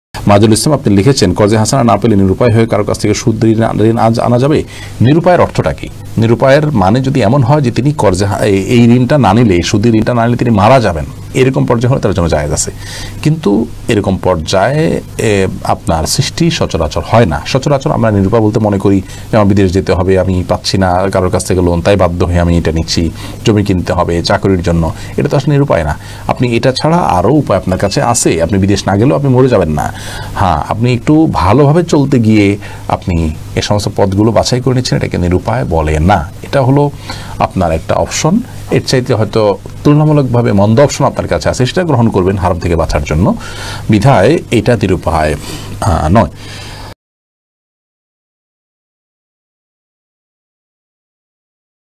সুদ ব্যতীত ঋণ না পেলে নিরুপায় হয়ে সুদ দিয়ে ঋণ আনা যাবে? Revival of Iman
শায়খ আহমাদুল্লাহ এর শরয়ী সমাধান থেকে সংগৃহীত:
কর্জে হাসানা (উত্তম ঋণ) না পেলে নিরুপায় হয়ে সুদ দিয়ে ঋণ আনা যাবে?